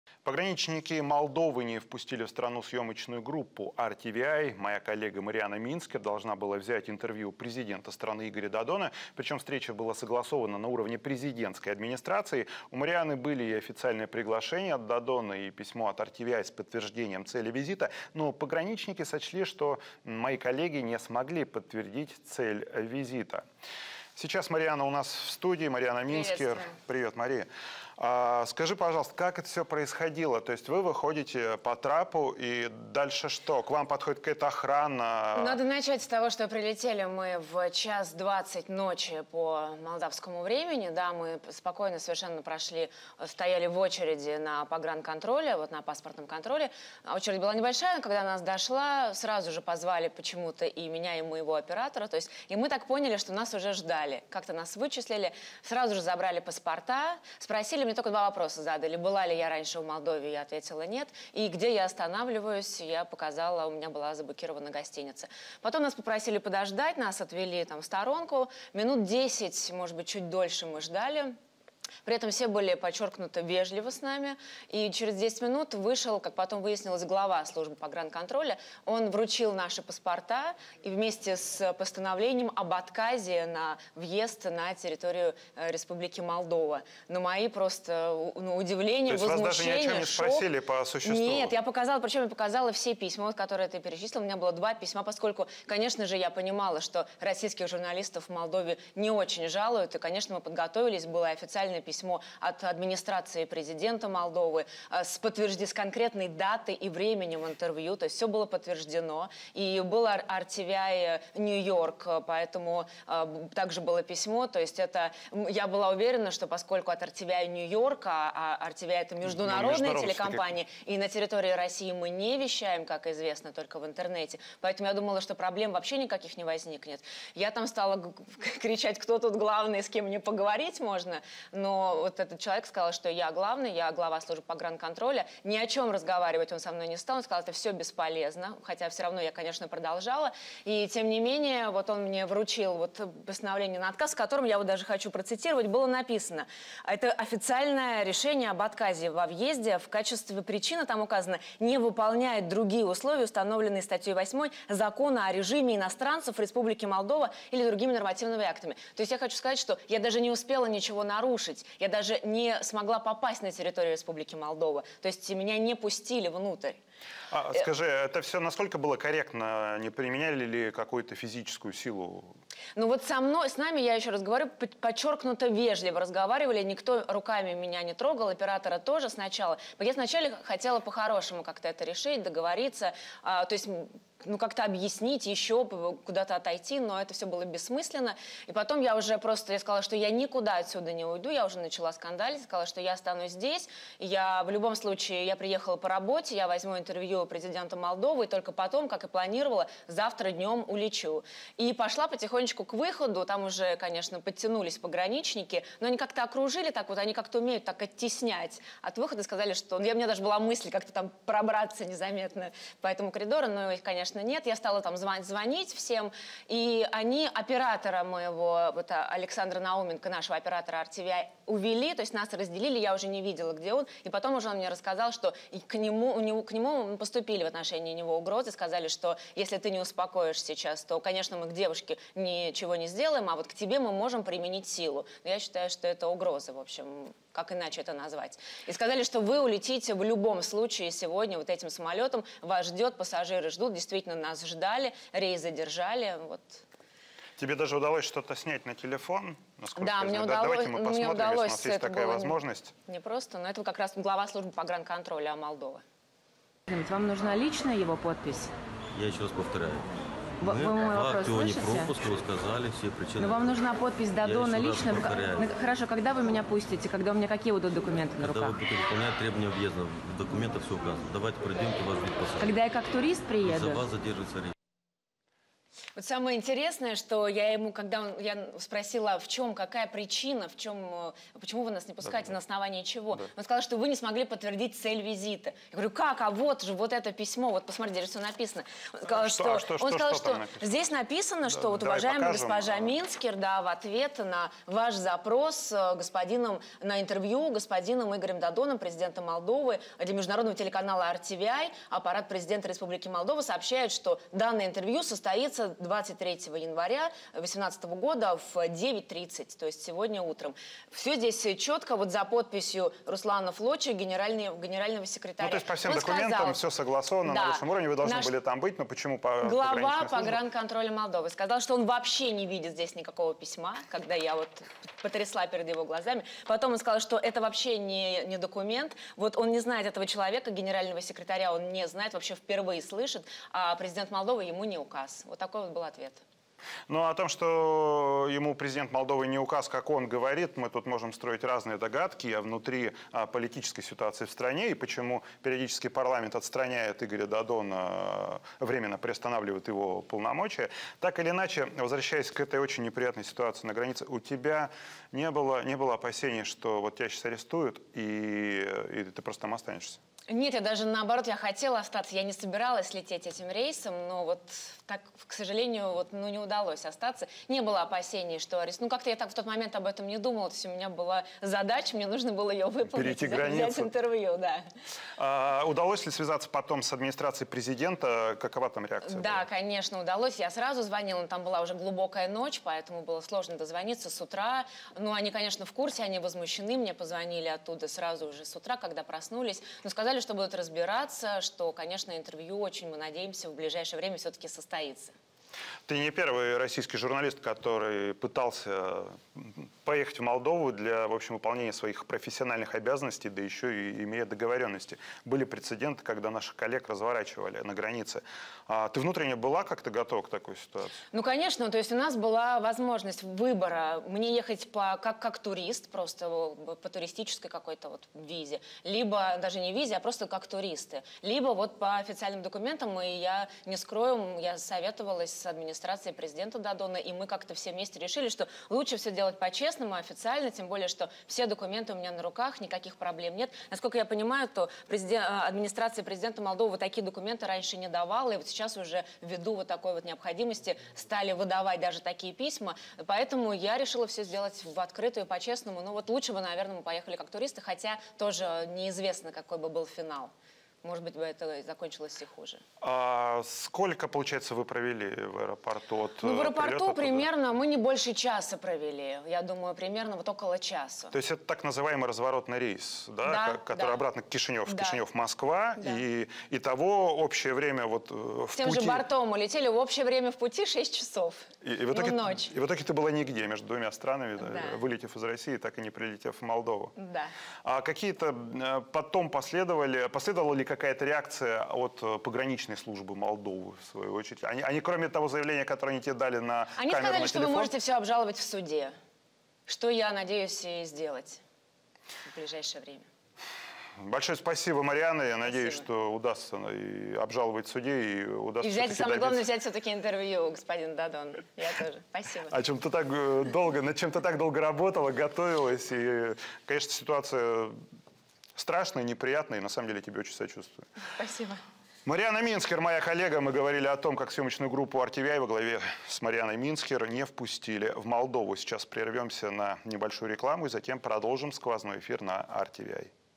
Журналист RTVI Марианна Минскер о конфликте в аэропорту Кишинева: «Мы поняли, что нас уже ждали»
23 января молдавские пограничники отказались пустить в страну журналиста RTVI Марианну Минскер. Она планировала взять интервью у главы республики Игоря Додона. Пограничники заявили, что не понимают цель визита. Обсудили ситуацию с Марианной Минскер в студии RTVI.